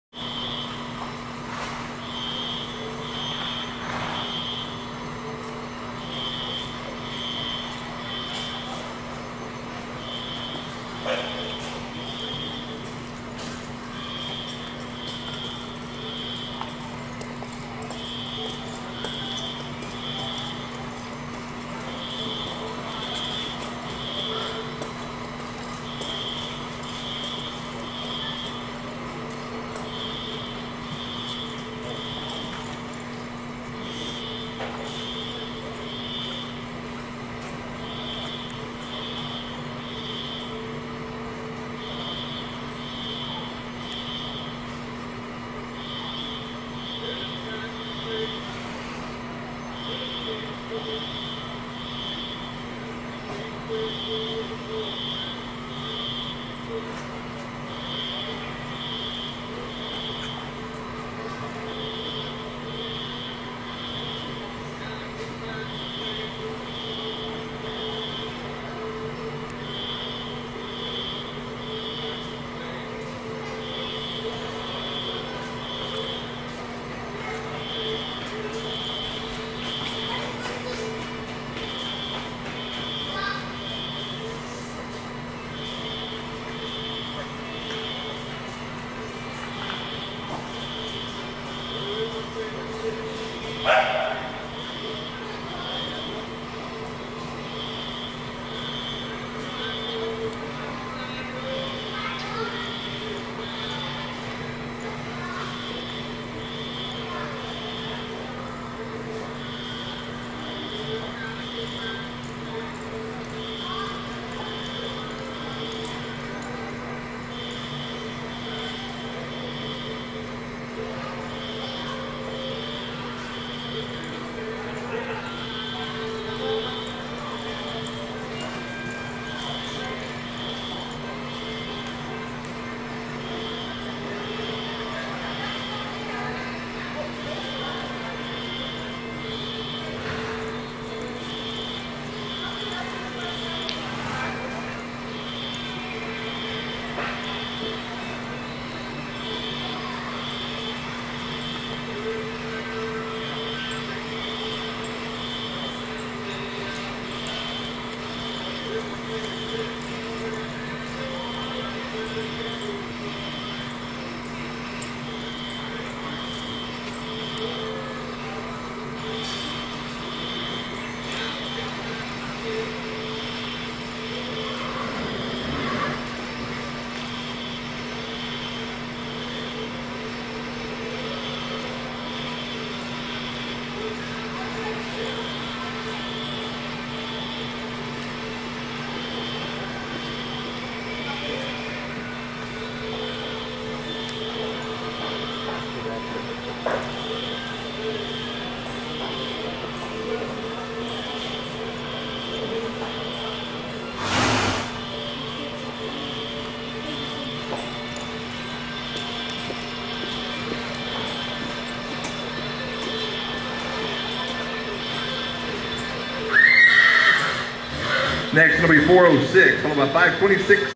Pistol Packin Chick
2007 Custom Pistol Gelding out of a daughter of Smart Chic Olena.  Lightly shown with huge stops and nice spins. Low headed. Big mane and tail. A great gelding.